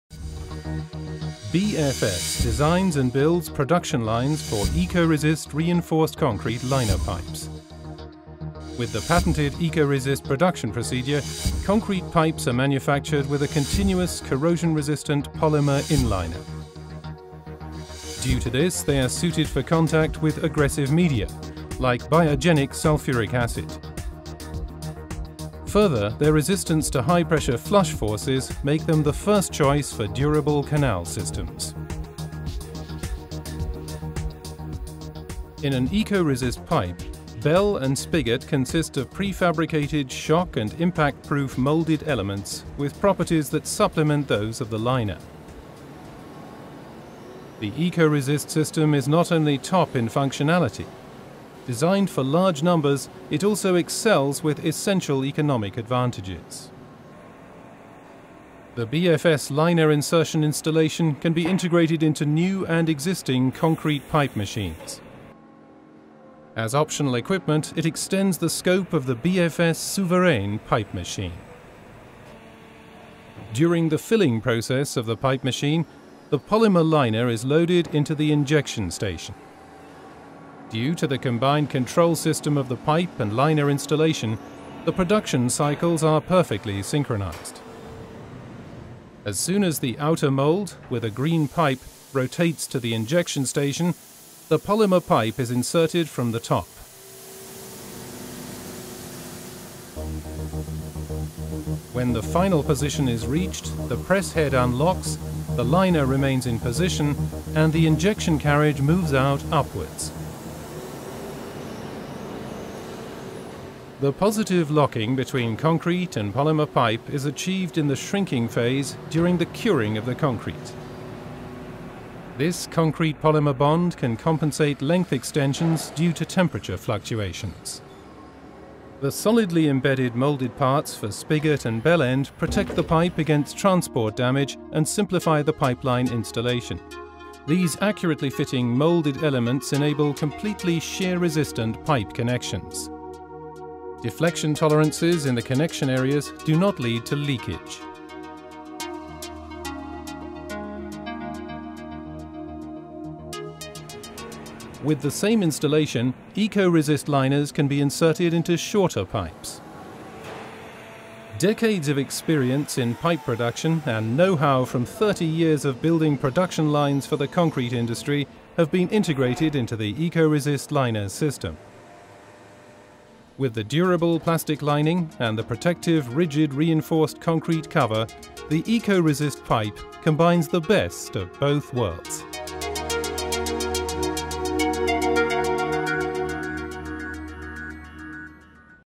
BFS Ecoresist Pipe Liner System (English)
For waste water applications, Afinitas recommends the innnovation of BFS Ecoresist. Designed for high productivity, the patented BFS Ecoresist System enables concrete pipes to be manufactured with a continuous corrosion-resistant polymer inner lining. This material can withstand the effects of aggressive media like biogenetic sulfuric acid and offers high pressure flush resistance and complete water tightness. These benefits, plus trusted BFS expertise and service, make Ecoresist the ideal solution for durable waste water lines.